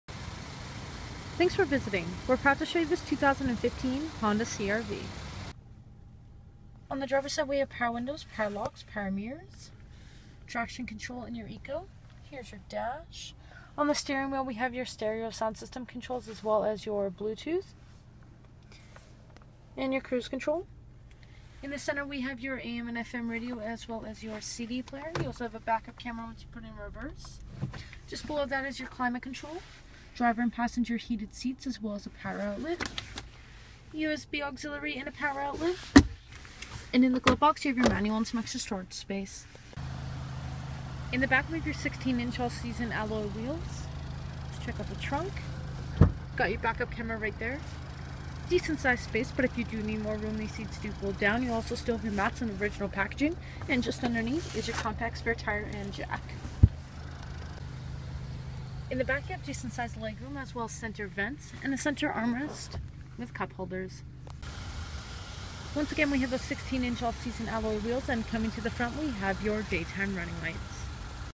2015 Honda CR-V AWD 5dr LX | Alberta Honda
This is a 2015 Honda CR-V AWD 5dr LX with CVT Transmission transmission White[NH677P,Aspen White Pearl] color and Black interior color.  This video is recorded and uploaded by cDemo Mobile Inspector